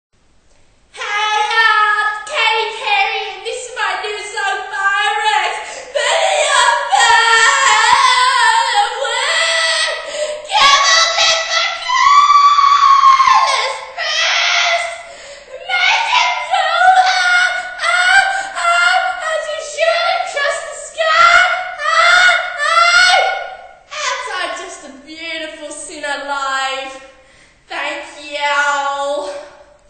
This is what Katy Perry sounds like live.
Katy Perry can't sing live.  thats all I have to say.